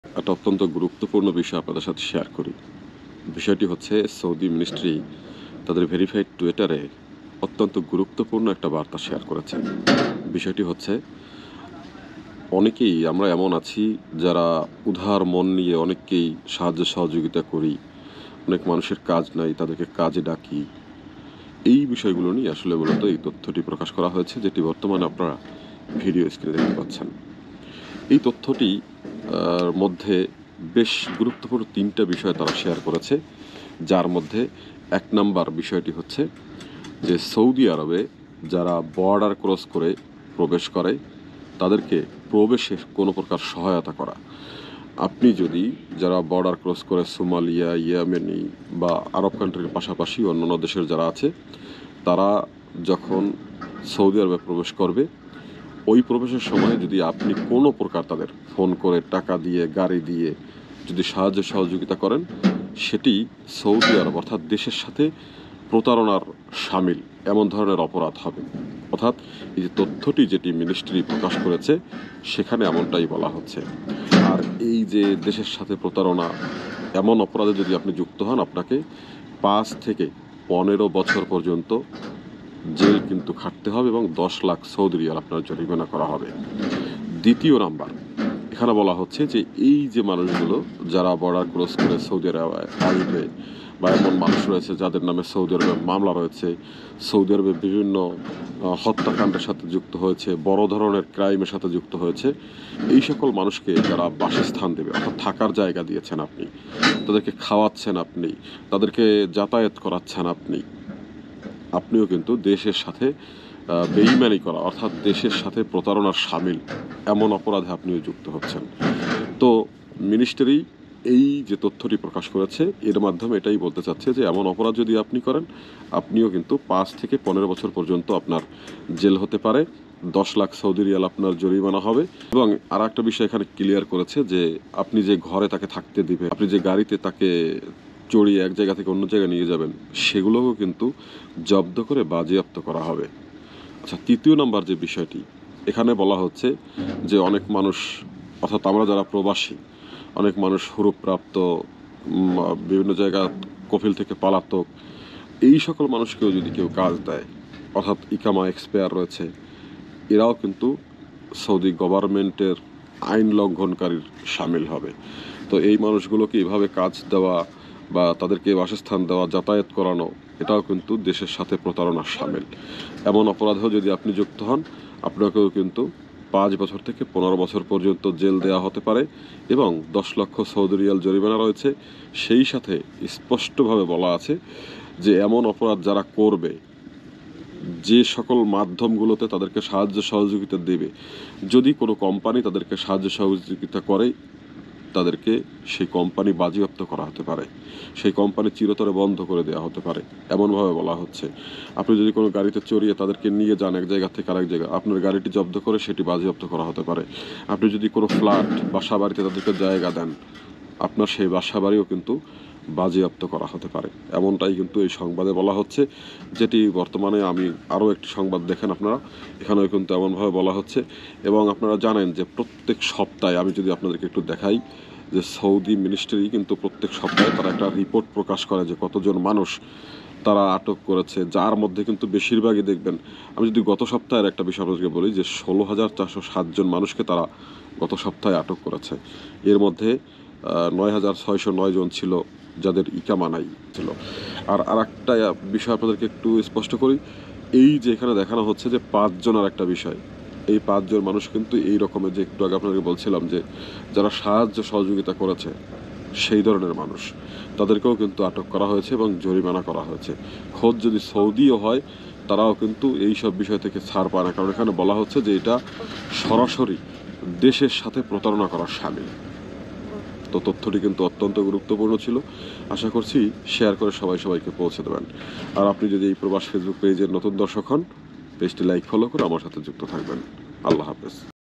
সৌদি আরব প্রবাসীরা, ছোট অজানা ভূলে ফেসে যেতে পারেন, হতে পারে ১৫ বছরের জেল।
সৌদি মিনিস্ট্রির দেয়া কঠিন আইন